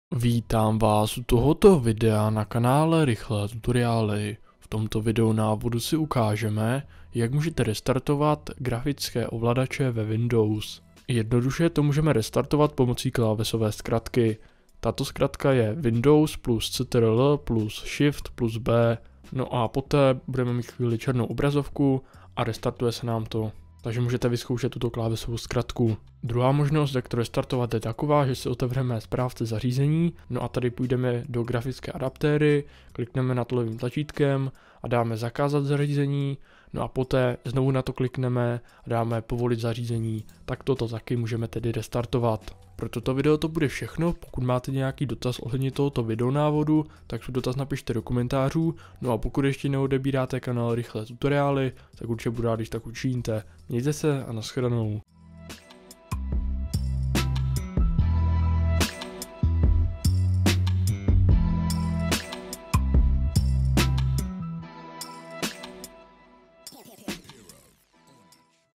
Jak ve Windows restartovat grafický ovladač - Jak restartovat grafickou kartu ve Windows (tutoriál)
V tomto video návodu si ukážeme, jak ve Windows restartovat grafický ovladač.

▷ Na tomto kanále naleznete různé naleznete edukativní videa nejen z prostředí Windows či Android... ◁